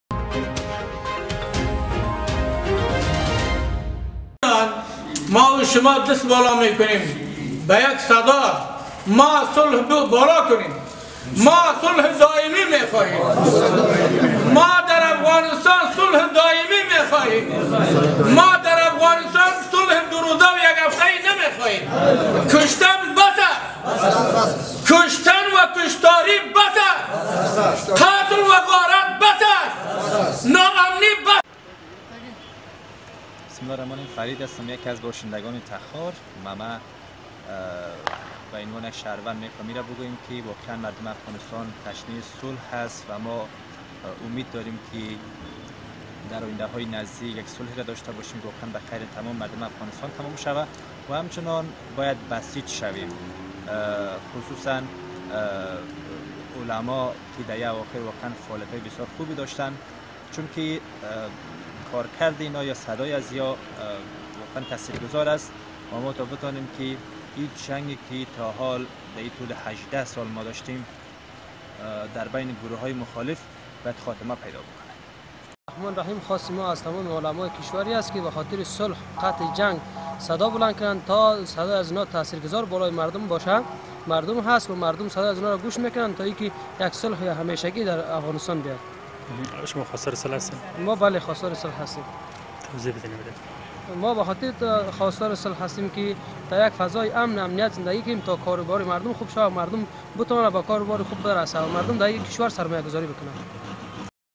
علمای دین در تخار طی یک گردهمای خواستار صلح شدند
آنان از گروه‌‌های مسلح به ویژه طالبان خواستند، دست از جنگ بردارند و مردم افغانستان از خشونت های جنگی نجات دهند.
عبدالباری راشد رییس ارشاد حج و اوقاف تخار می‌گوید: جنگ به اندازه کافی برای مردم افغانستان نگرانی خلق کرده است
وی می‌گوید: کشته شدن هزاران نفر از شهروندان  افغانستان  و تخریب بخش بزرگ از زیر ساخت های کشور حاصل آن است که ادامه آن برای مردم کشور قابل پذیرش نیست.